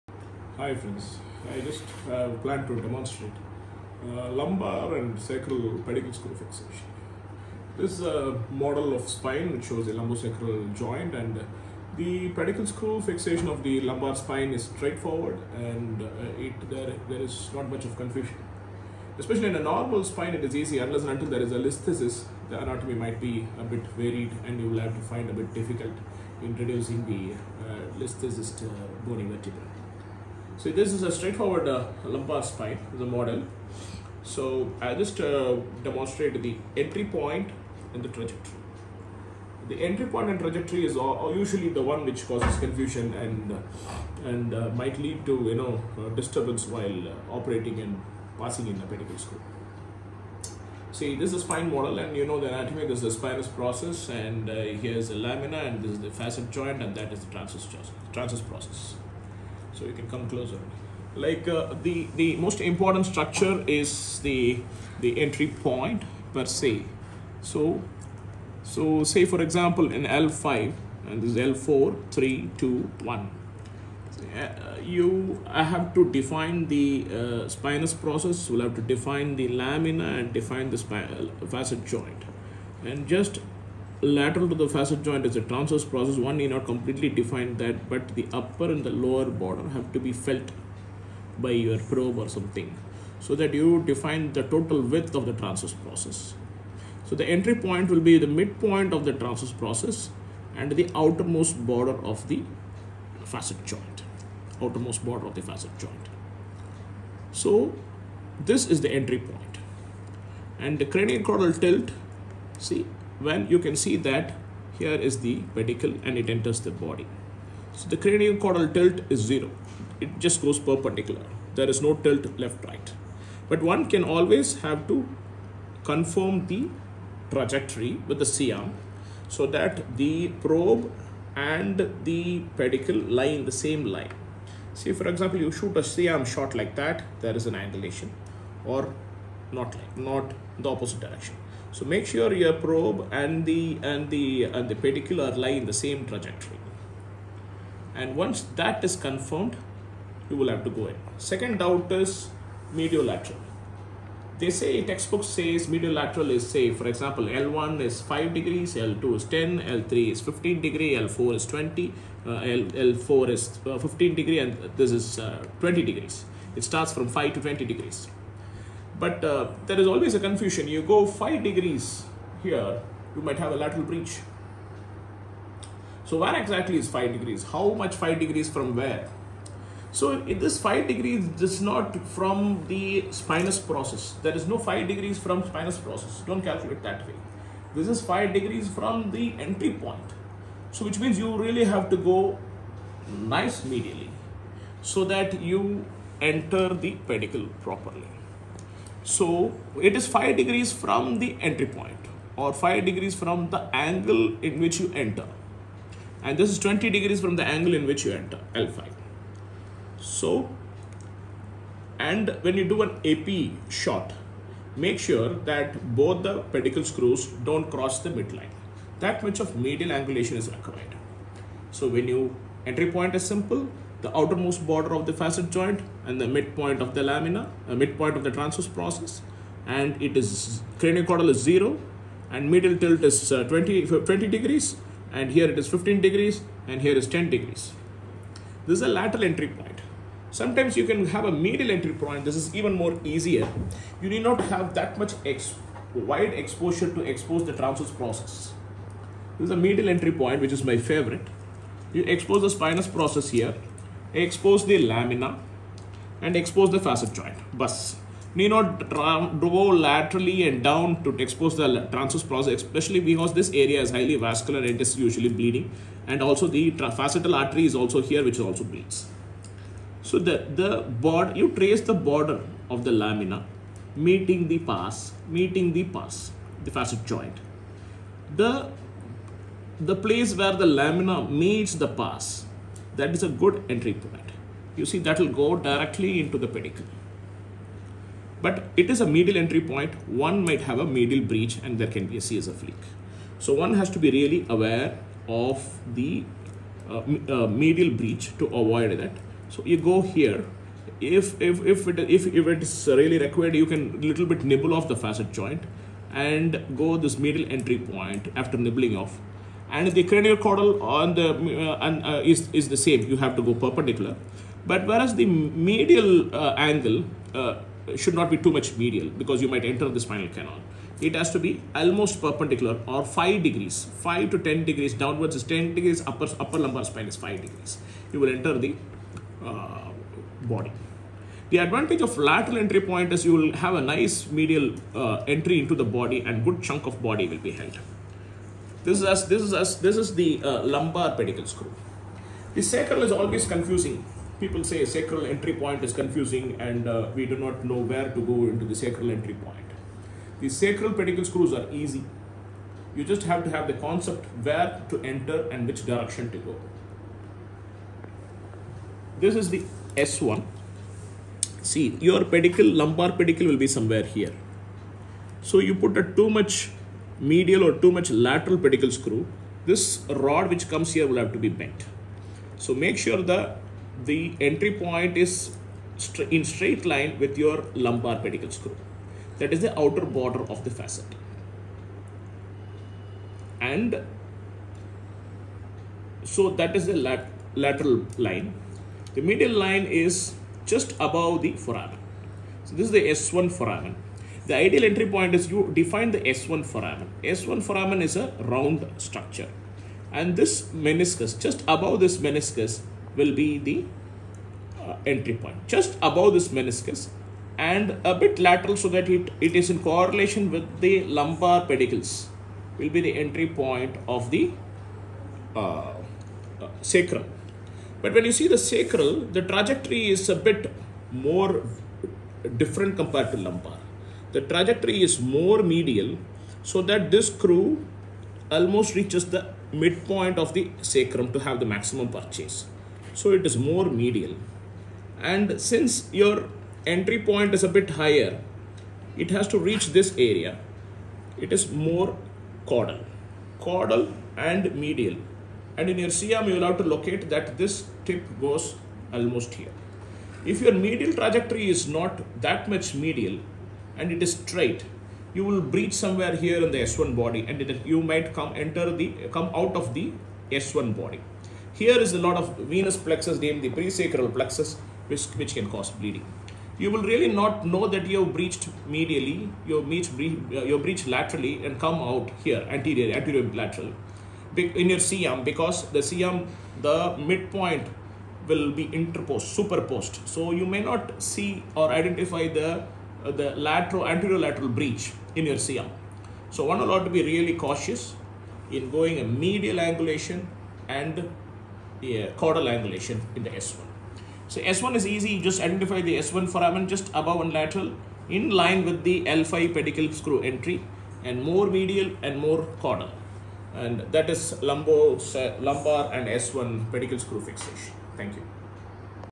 Pedicle Screw technique of the lumbar spine.
This video demonstrates the lumbar spine instrumentation technique, and the pedicle screw insertion - the point of entry and it’s trajectory. Dr. Ganesh kumar - brain & spine surgeon